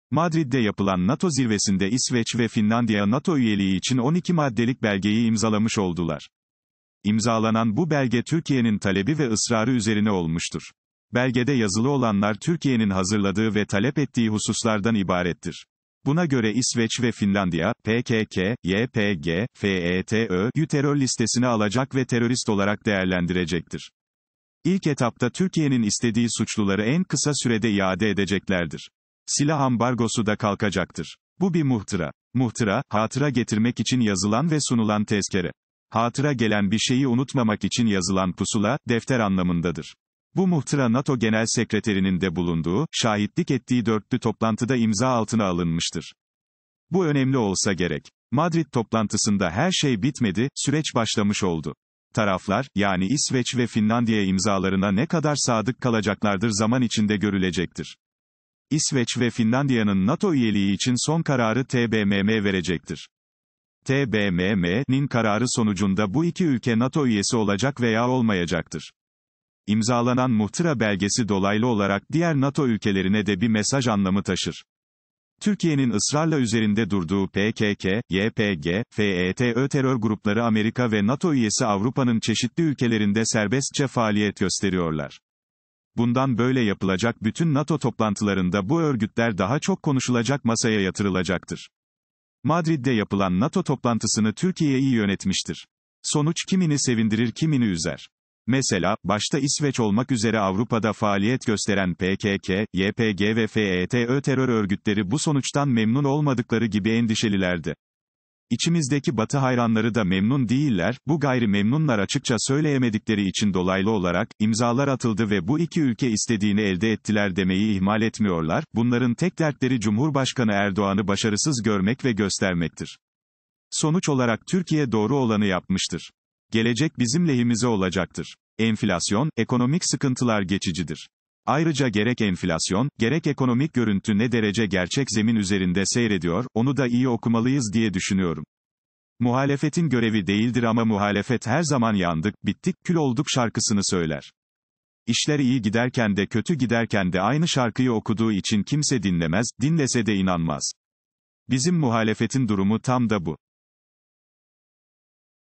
İçimizdeki Batıcılar neden üzüldü
Niyazi Pakyürek - İçimizdeki Batıcılar neden üzüldü - 03 Temmuz 2022 Pazar - Madrid’de yapılan NATO zirvesinde İsveç ve Finlandiya NATO üyeliği için 12 maddelik belgeyi imzalamış oldular.
İmzalanan bu belge Türkiye’nin talebi ve ısrarı üzerine olmuştur. Belgede yazılı olanlar Türkiye’nin hazırladığı ve talep ettiği hususlardan ibarettir.
Buna göre İsveç ve Finlandiya, PKK, YPG, FETÖ’ yü terör listesine alacak ve terörist olarak değerlendirecektir. İlk etapta Türkiye’nin istediği suçluları en kısa sürede iade edeceklerdir. Silah ambargosu da kalkacaktır.
Bu bir muhtıra. Muhtıra, hatıra getirmek için yazılan ve sunulan tezkere. Hatıra gelen bir şeyi unutmamak için yazılan pusula; defter anlamındadır. Bu muhtıra NATO genel sekreterinin de bulunduğu, şahitlik ettiği dörtlü toplantıda imza altına alınmıştır. Bu önemli olsa gerek.
Madrid toplantısında her şey bitmedi, süreç başlamış oldu. Taraflar, yani İsveç ve Finlandiya imzalarına ne kadar sadık kalacaklardır zaman içinde...